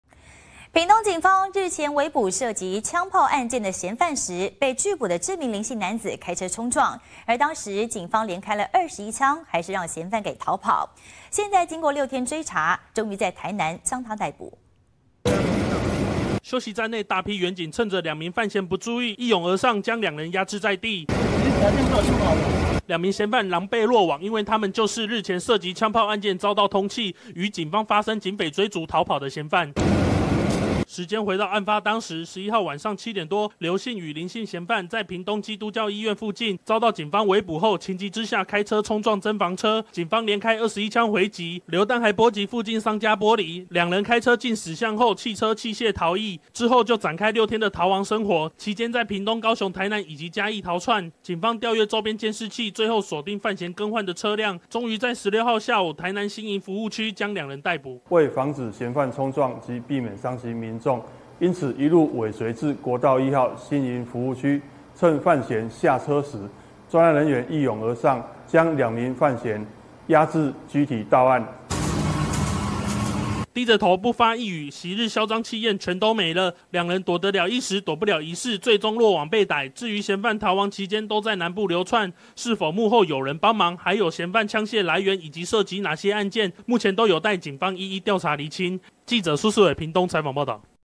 2槍嫌南部逃竄6天 國道新營服務區遭逮
屏東警方日前圍捕涉及槍砲案件嫌犯時，遭拒捕林姓男子開車衝撞，警方連開21槍還是讓嫌犯逃跑，經過6天追緝，終於在台南新營服務區將兩名犯嫌逮捕歸案，據了解兩人逃亡期間，都在南部地區流竄，背後是否有人幫忙，目前還有待釐清。